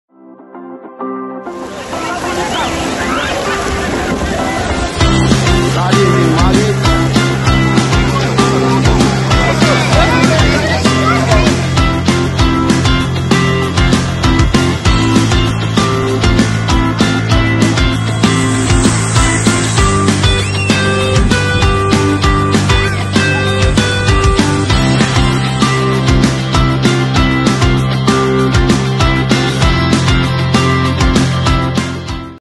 The USCT KZN South Coast Sardine RUN 2022
The silverfish are here! Watch the sardine run or greatestshoalonearth at kznsouthcoast in person, where there is plenty to seaanddo! Extendyourstay. videofootage compliments of the KZN Sardine Watch 2022.